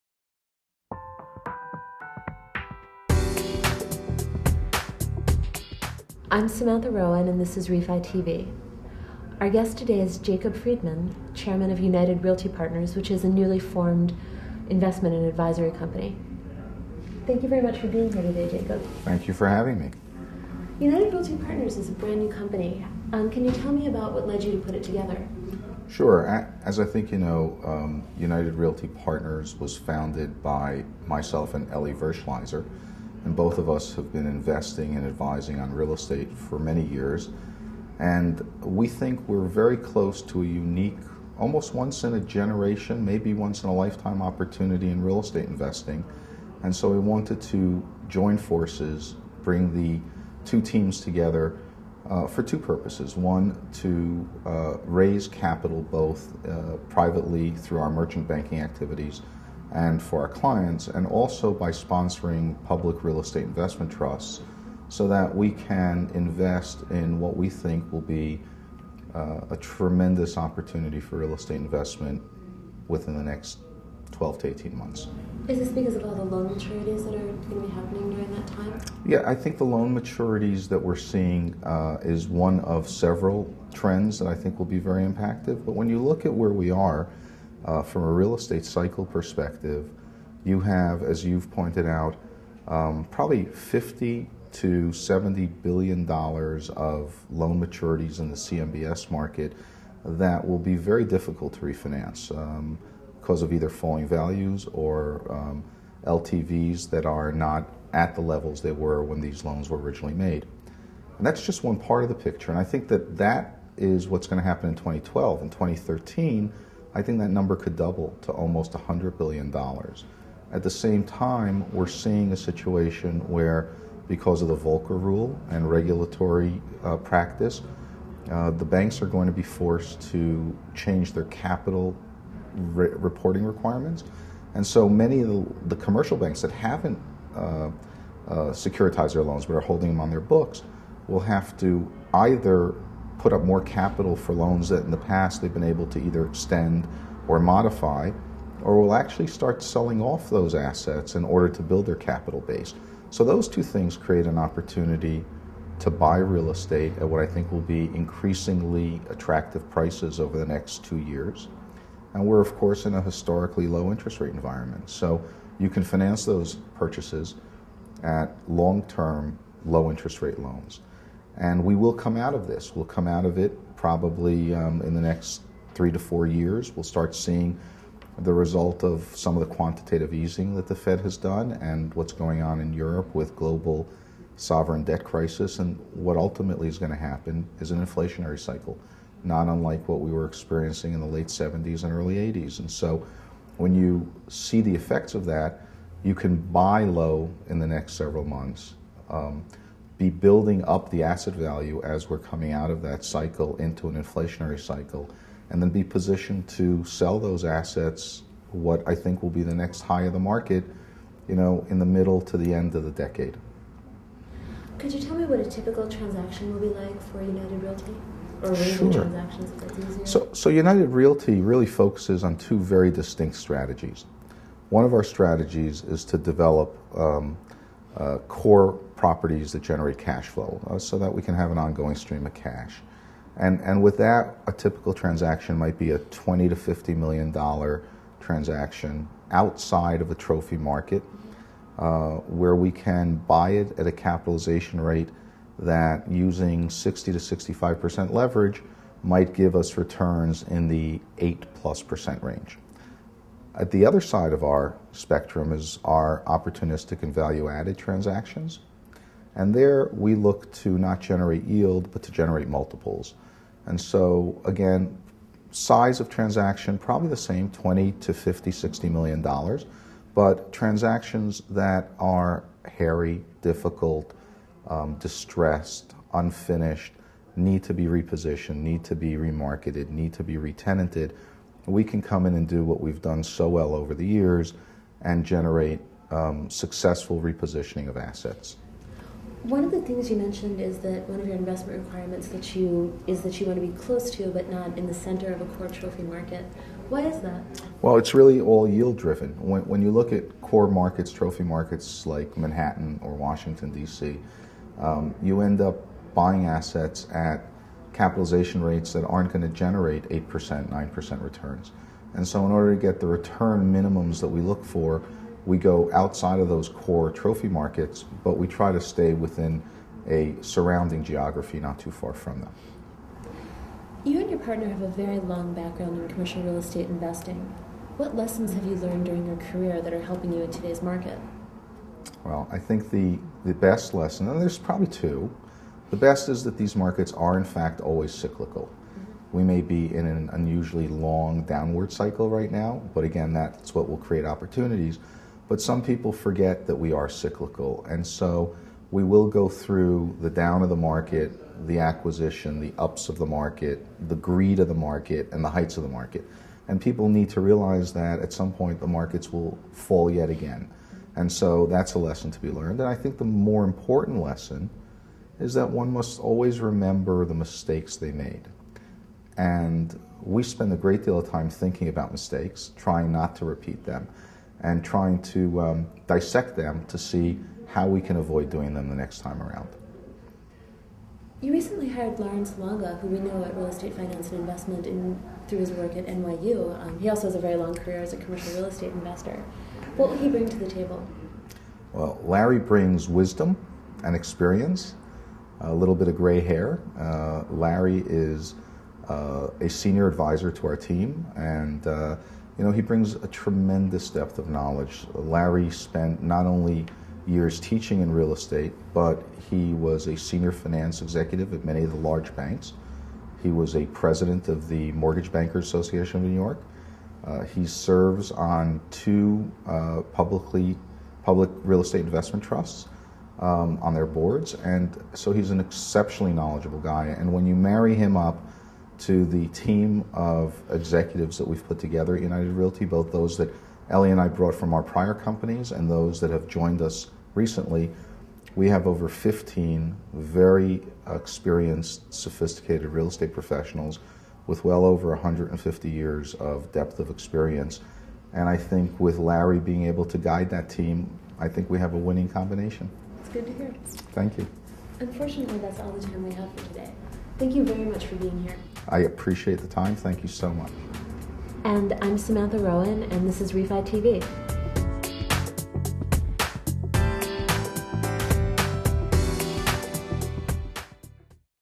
Jacob Frydman Visits REFI TV To Discuss United Realty Partners' Plans For 2012
Jacob Frydman, CEO of United Realty Partners visits Real Estate Finance Intelligence Television to discuss plans for 2012. Video courtesy of Real Estate Finance Intelligence